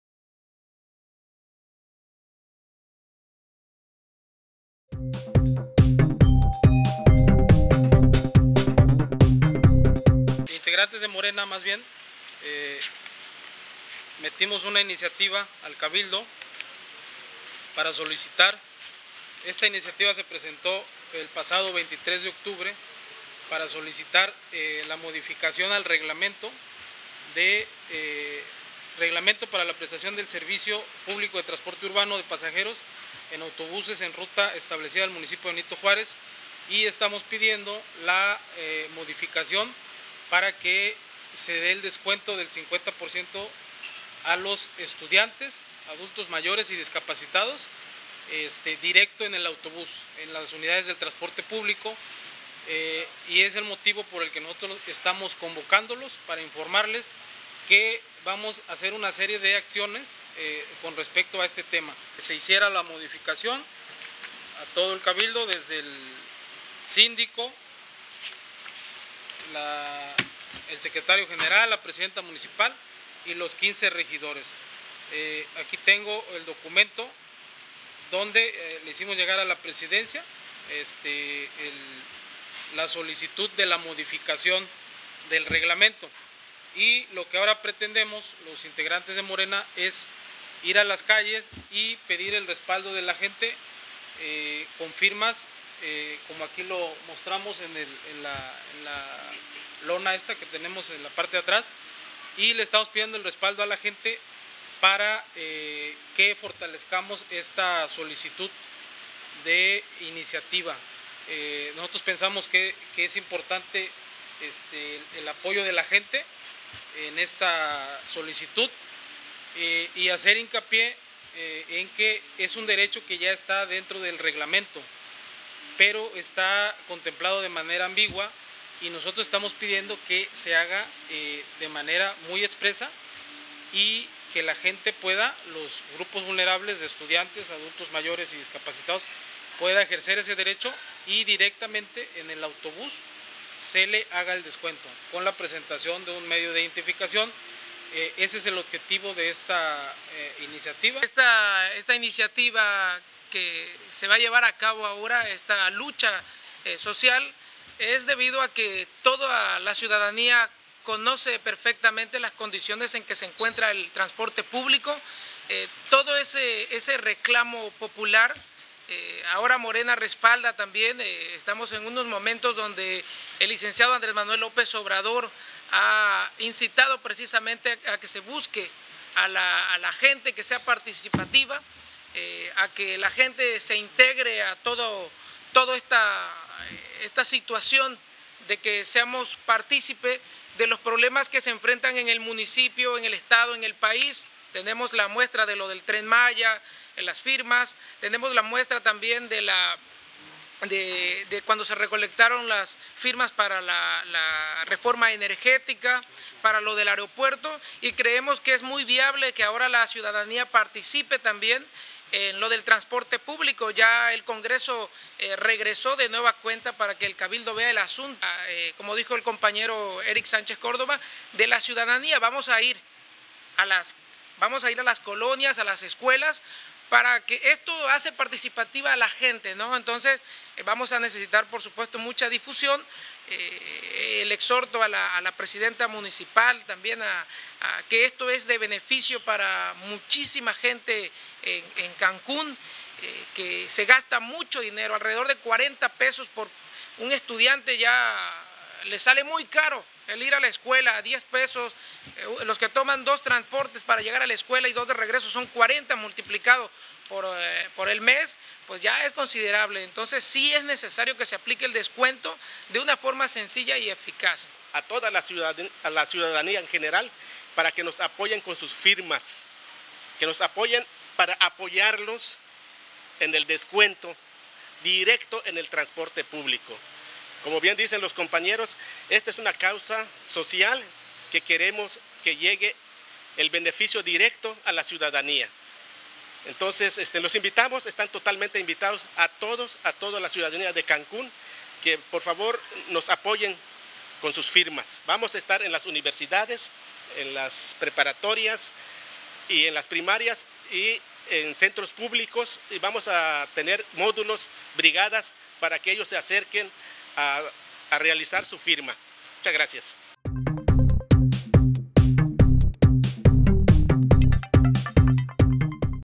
EXIGEN DESCUENTO DIRECTO DE 50% EN EL TRANSPORTE PÚBLICO
Militantes de Morena recopilarán firmas de apoyo en diversas partes de Cancún para solicitarle al ayuntamiento que las concesionarias de transporte realicen descuento del 50% directo a estudiantes, adultos mayores y discapacitados.